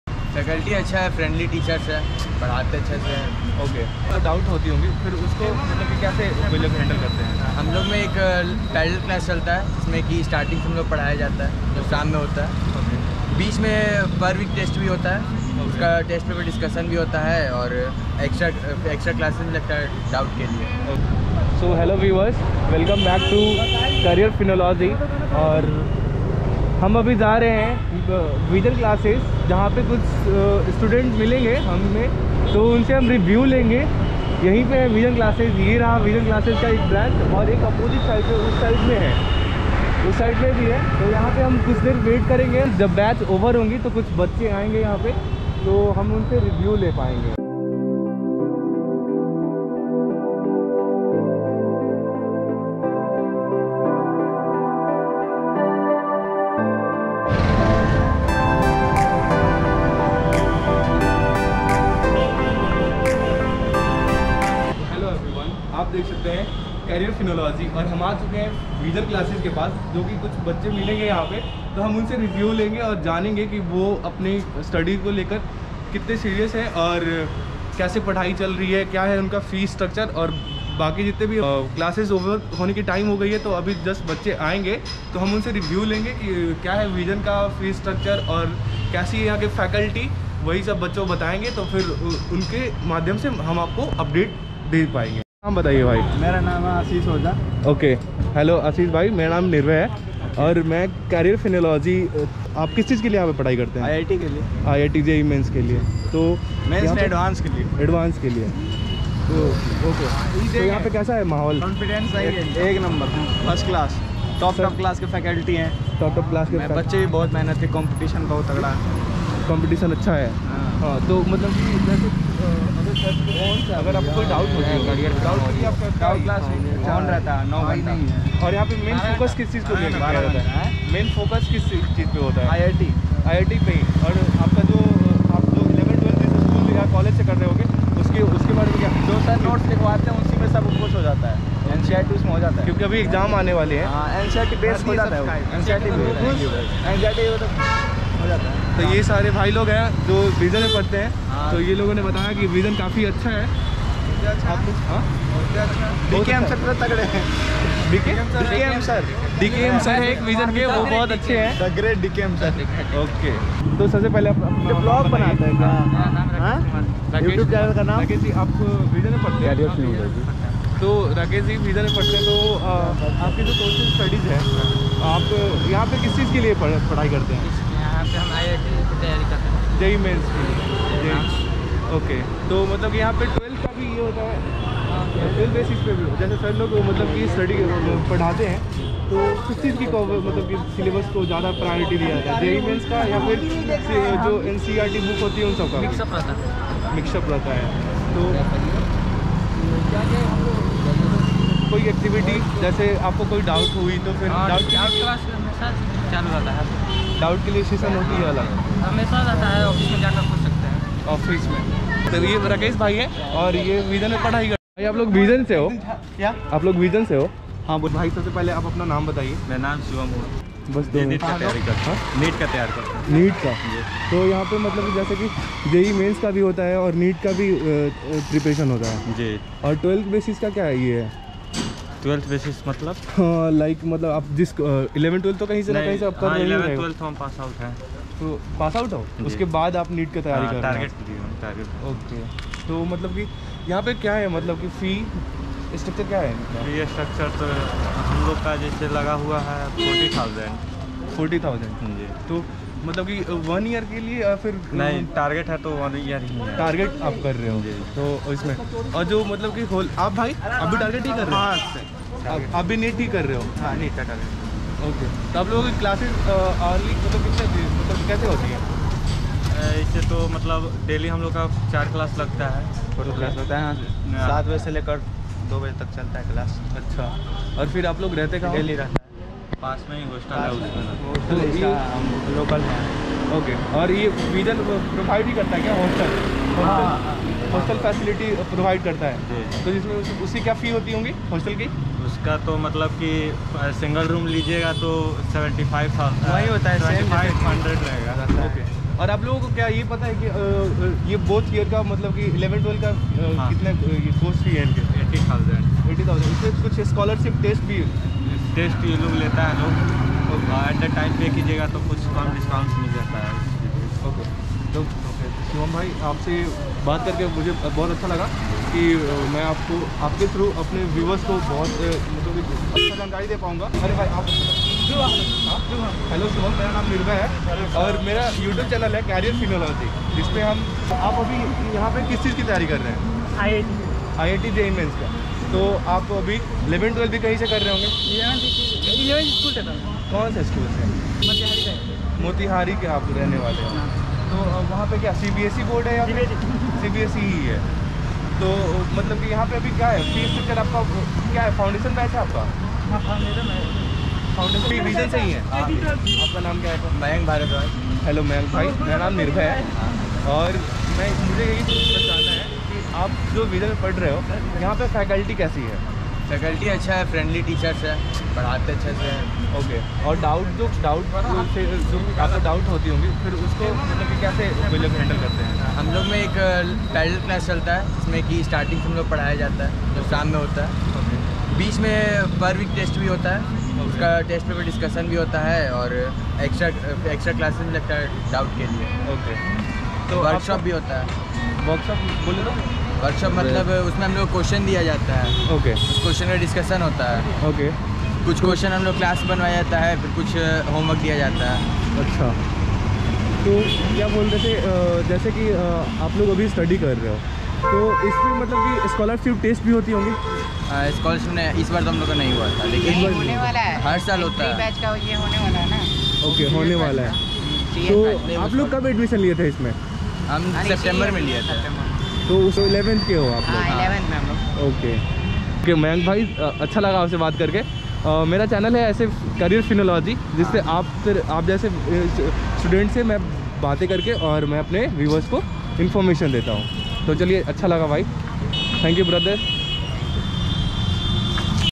Vision Institute Honest Review by Students in Boring Canal Road Patna | Career Finology | IIT | NEET
I am going to show you Vision Institute Honest Review by Students in East Boring Canal Road Patna.

Any Query. @CareerFinology

Address: Jay Kamla Plaza, E Boring Canal Rd, near Hotel Lalita, Sri Krishna Puri, Patna, Bihar 800001